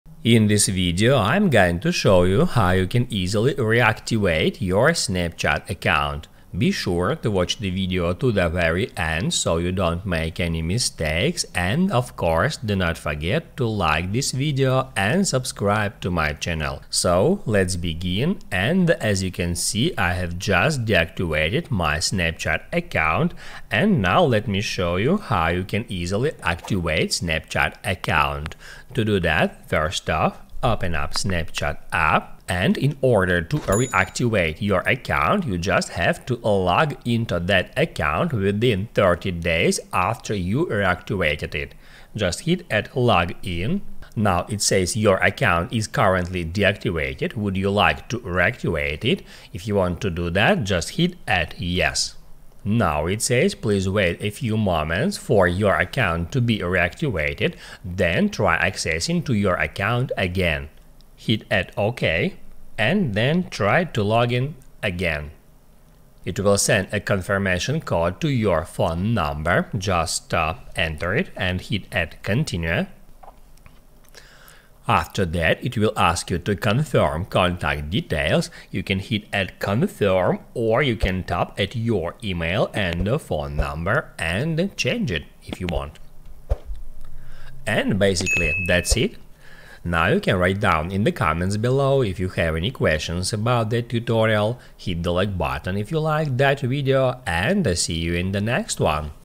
How to Reactivate Snapchat Account
If you are looking for a video about how to reactivate snapchat account, here it is!
In this video I will show you how to reactivate snapchat account before 30 days. Be sure to watch the video to the very end.
You will learn how to reactivate snapchat account after deactivating. That's easy and simple to do from your phone.

00:00 - Intro
00:24 - How to reactivate snapchat account